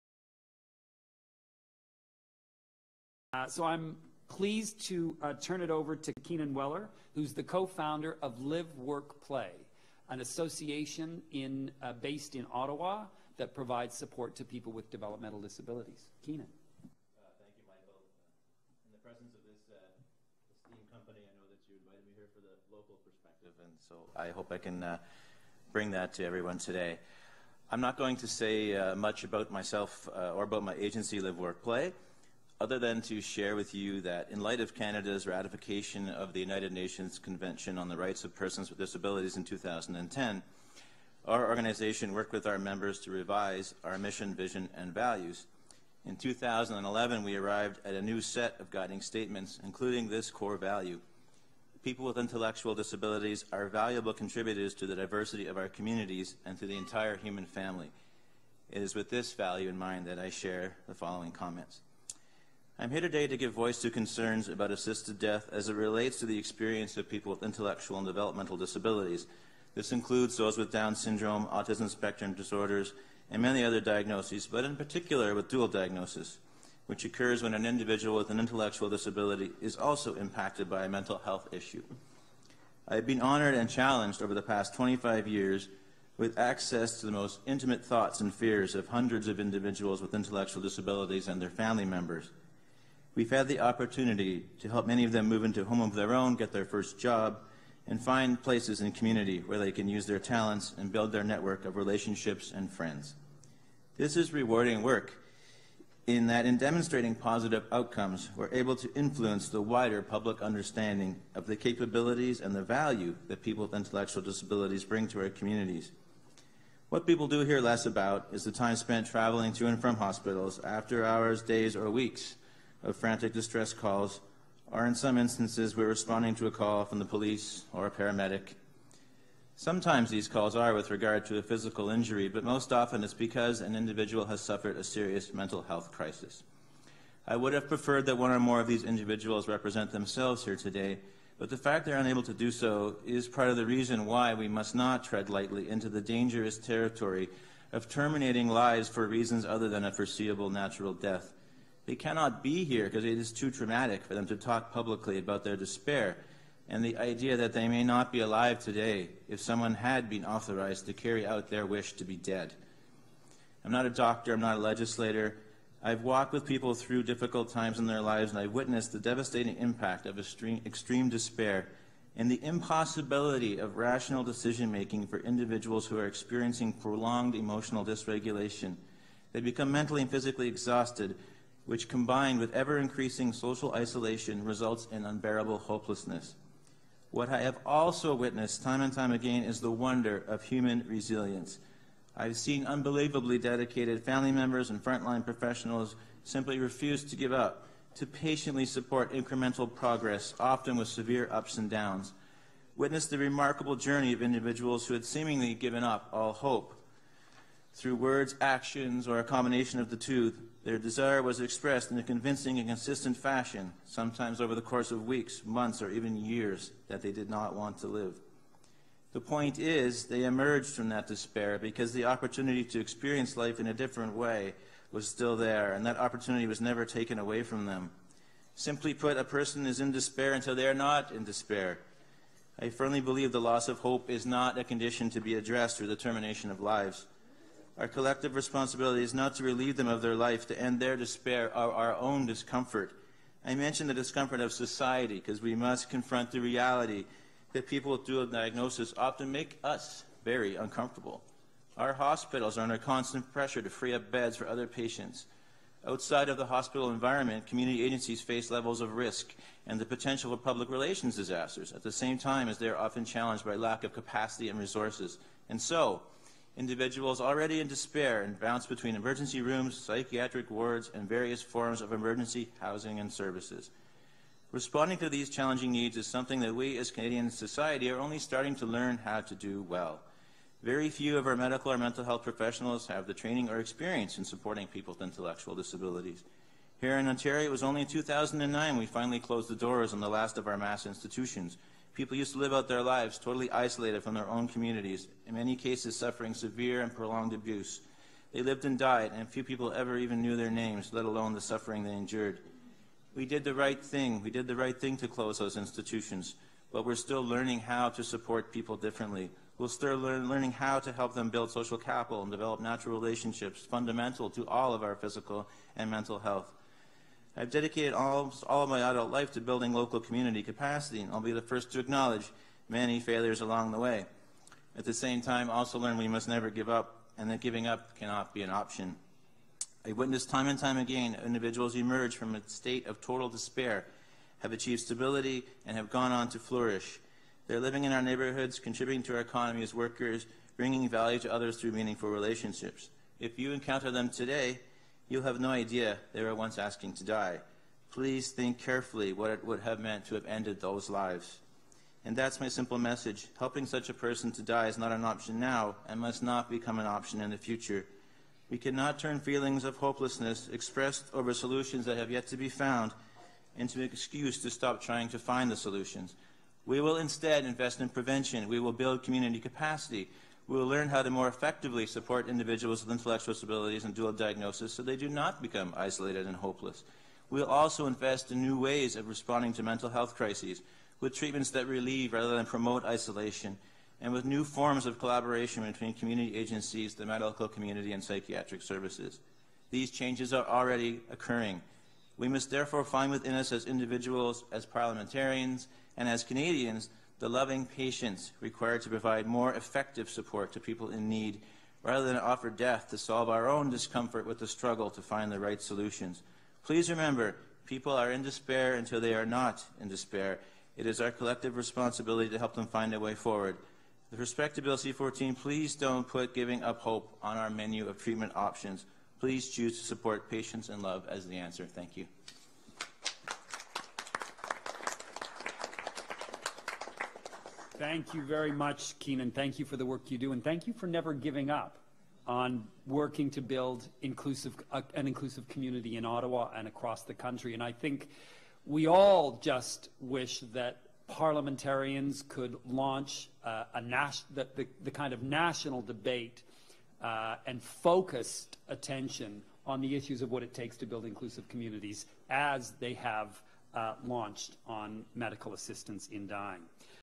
Share Your Voice: Community Forum for Robust Safeguards in Bill C-14 (June 16, 2016)
This video clip is limited to the comments of LiveWorkPlay Co-Leader Keenan Wellar. A link to access the entire video can be found at the conclusion of this clip (90 minutes).

Note: The Honourable Minister Carla Qualtrough spoke at the conclusion of the event.

Welcome:  James Hicks, National Coordinator, Council of Canadians with Disabilities

Moderator: Michael Bach, Executive Vice-President, Canadian Association for Community Living

Speakers:
• Catherine Frazee, Professor Emeritus Ryerson University School of Disability Studies
• Ellen Cohen, National Coordinator, National Network for Mental Health
• Bonnie Brayton, National Executive Director, DAWN-RAFH Canada
• Wendall Nicholas, Chair, Wabanaki Council on Disability
• Keenan Wellar, Co-Founder, LiveWorkPlay Ottawa
• Jennifer Johannesen, ethicist, parent and patient advocate
• Amy Hasbrouk, Toujours Vivant-Not Dead Yet
• Dr. Harvey Chochinov, Canada Research Chair in Palliative Care